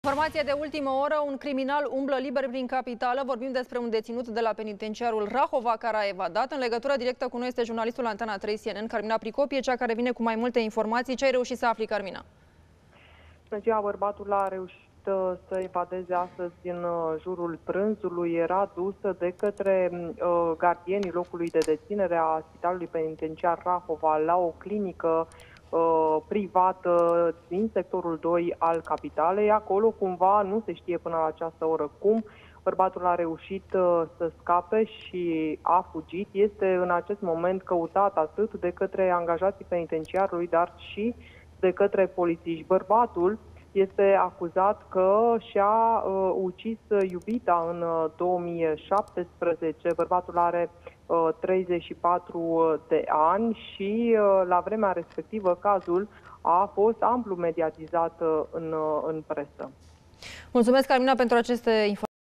Un bărbat condamnat la închisoare pentru omor a evadat în Capitală
Un bărbat în vârstă de 34 de ani, condamnat pentru omor, a evadat în Capitală.